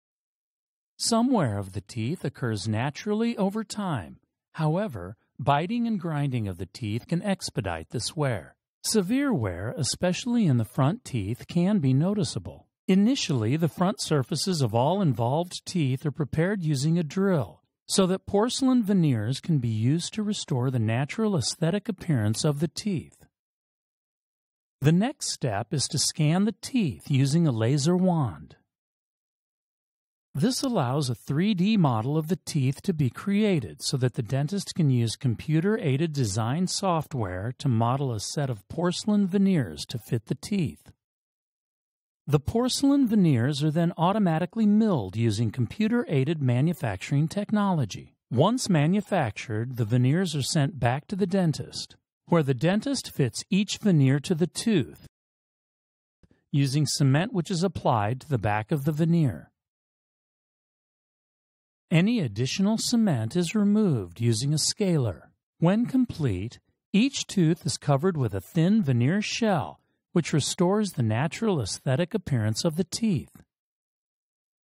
Veneers: Worn, Porcelain CAD/CAM
Some wear of the teeth occurs naturally over time, however, biting and grinding of the teeth can expedite this wear.